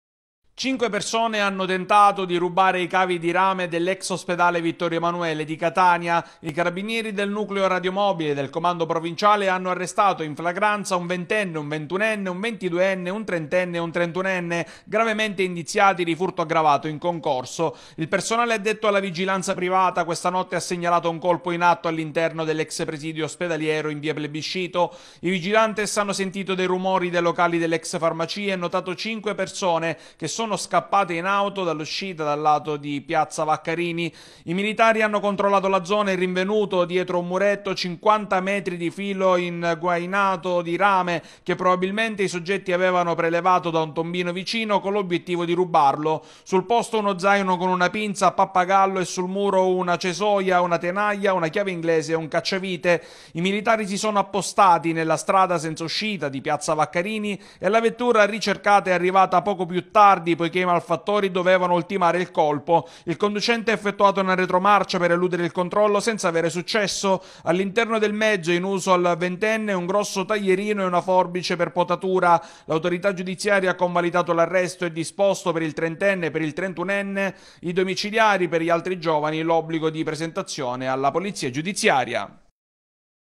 Catania, tentano furto cavi di rame da ex ospedale Vittorio Emanuele: 5 arresti
Cinque persone arrestate in flagranza dai Carabinieri del Nucleo Radiomobile del Comando provinciale di Catania: hanno tentato di rubare i cavi di rame dall'ex ospedale Vittorio Emanuele di via Plebiscito.